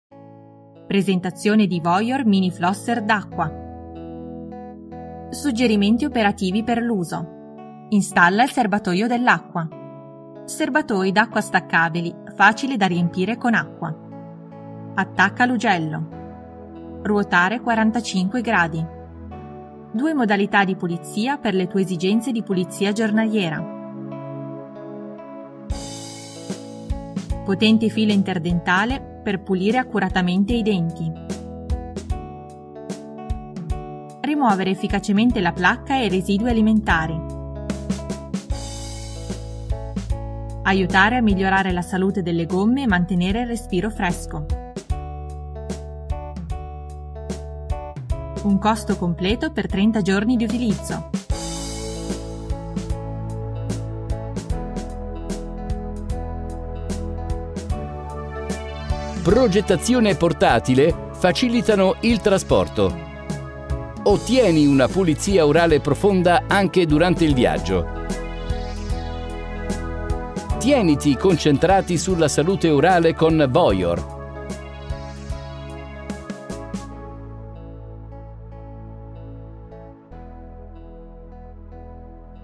Più piccolo e più facile da trasportare. Idropulsore portatile VOYOR WF500
Un filo interdentale portatile per mantenere i tuoi denti sempre sani.